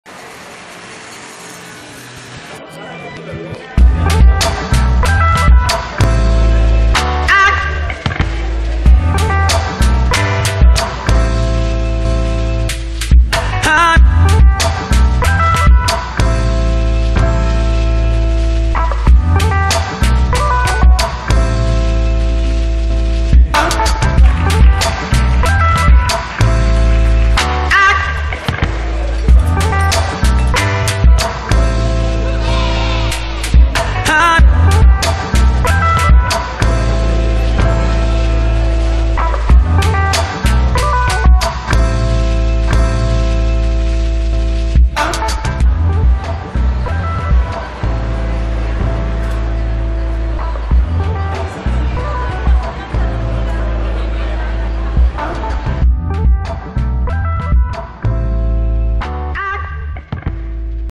Artista: Filomeno Machado. Exposición de grabados y obras
Inauguración de la exposición de Filomeno Machado en el Centro Ecosociocultural Miguel Hernández de La Zubia (Granada).